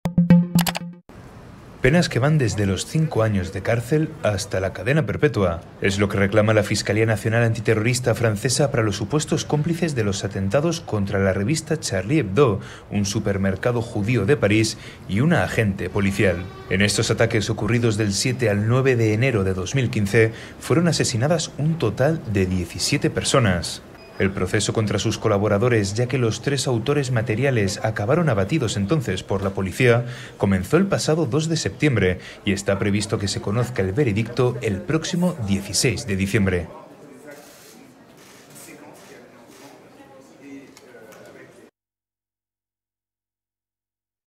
La Fiscalía pide penas de 5 años a perpetuidad en el juicio de Charlie Hebdo
La Fiscalía Nacional Antiterrorista francesa reclamó este martes penas que van desde los cinco años de cárcel hasta la cadena perpetua para los supuestos cómplices de los atentados del 7 al 9 de enero de 2015 contra la revista satírica Charlie Hebdo, un supermercado judío de París y una agente policial.

¡Visítenos!